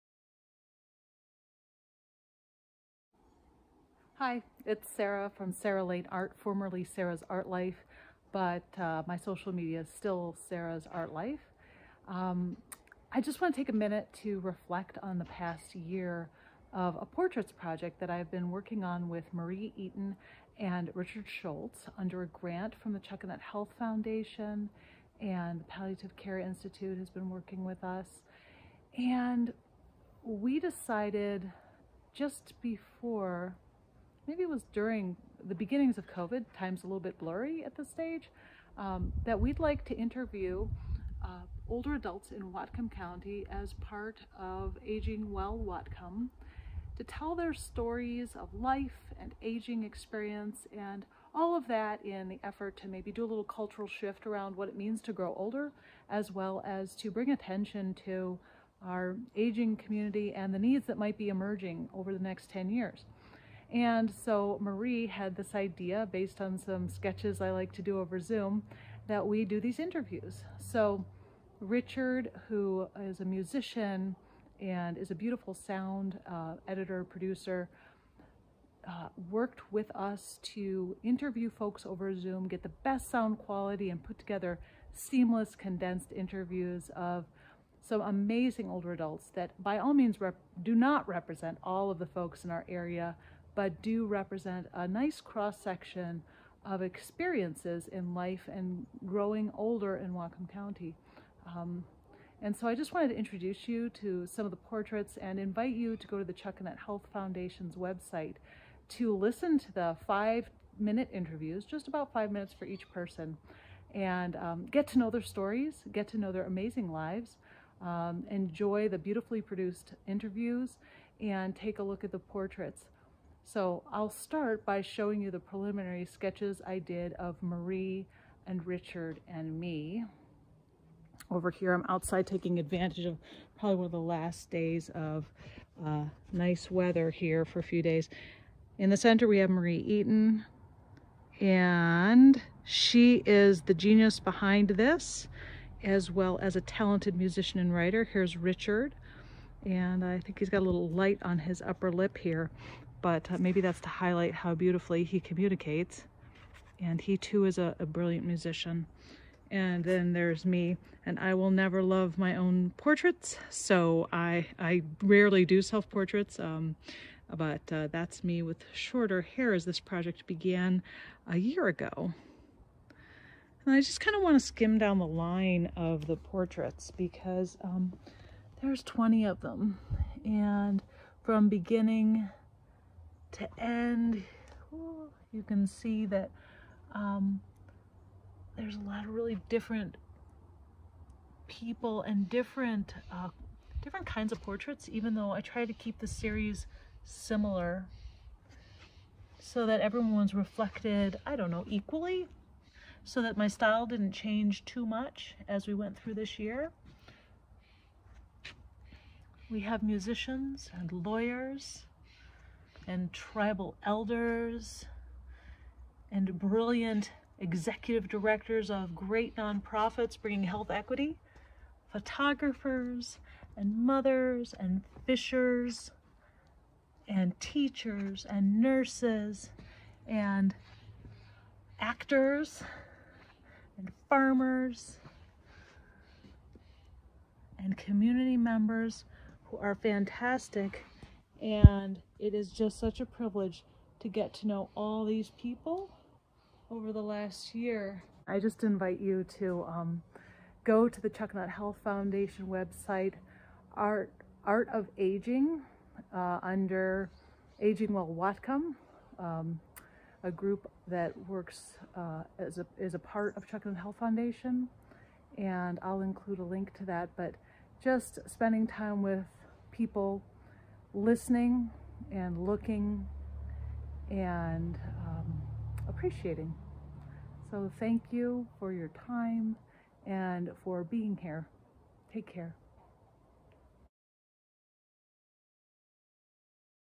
The Art of Aging - 20 Portraits and Interviews
A quick look at a portraits project completed in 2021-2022 via Zoom.